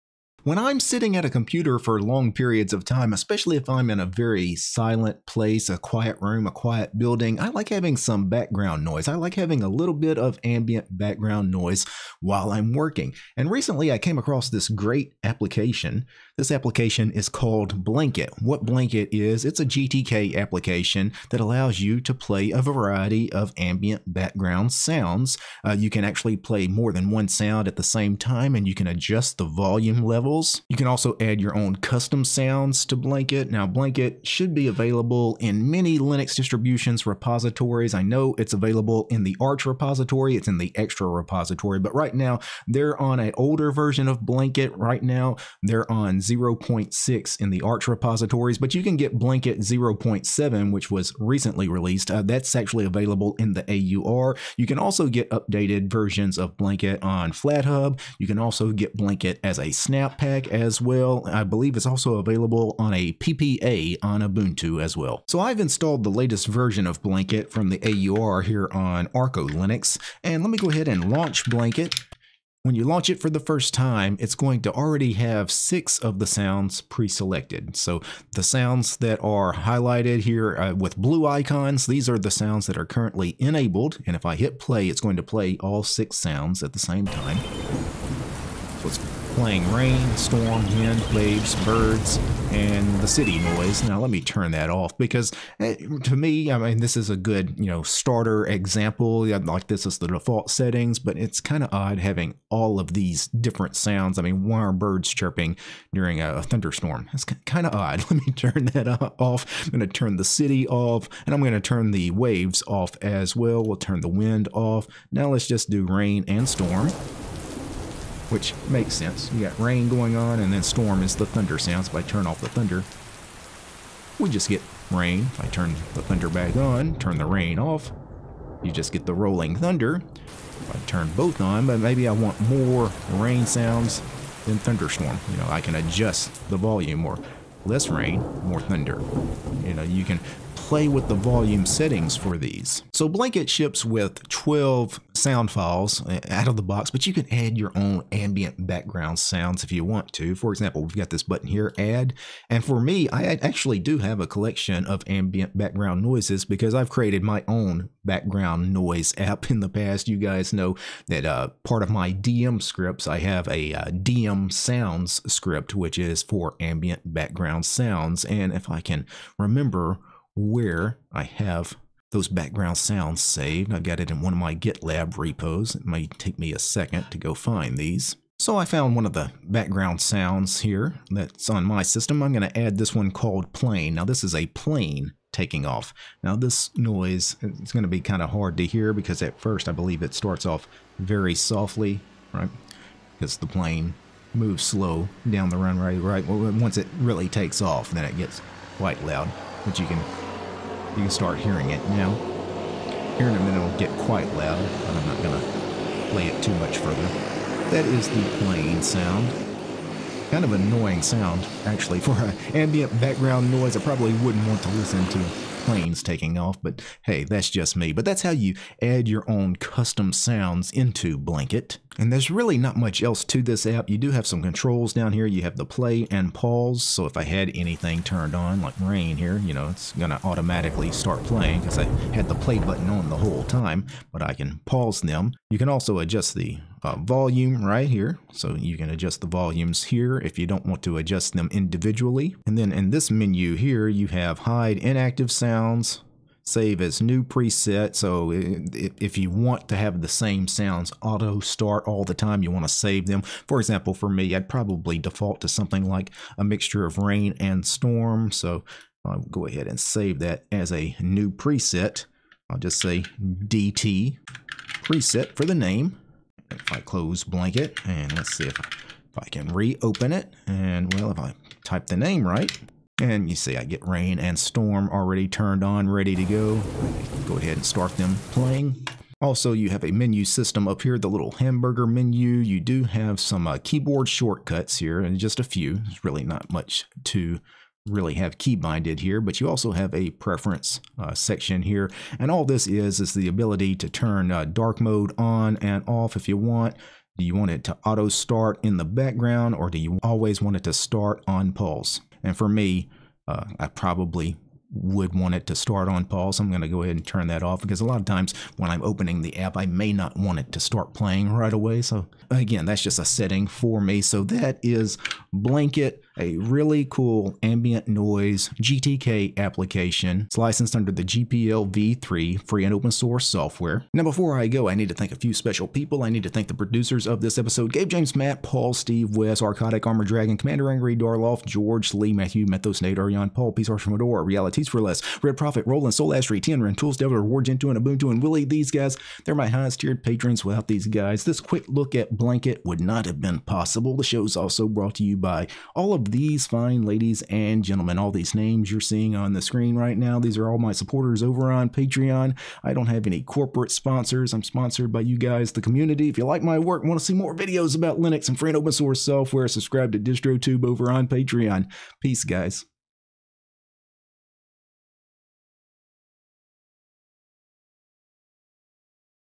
Blanket Is A Slick Little Ambient Sound Player
Blanket is a neat little GTK app that plays ambient background sounds.  You can improve focus and increase your productivity by listening to different sounds. It may also allow you to fall asleep in a noisy environment.